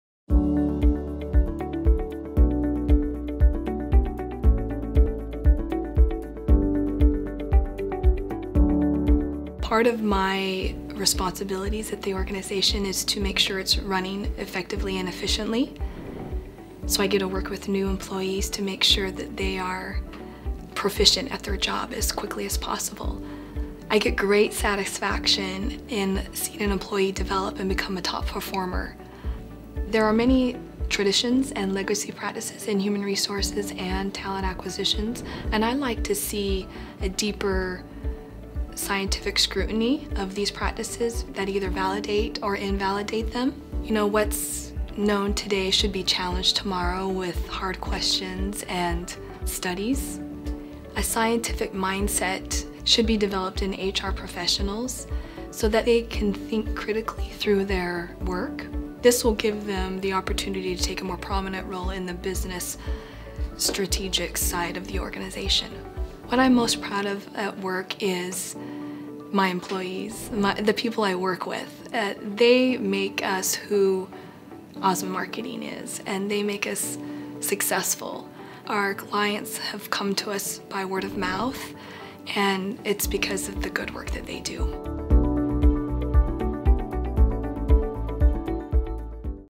Emily Woll is named HR Professional of the Year 2020 by Utah Business
Emily Woll, COO at Osmond Marketing, is named HR Professional of the Year 2020 by Utah Business Magazine. Watch as Emily explains how her role has changed with the evolving needs of today's customer and why maintaining a positive work culture is a priority.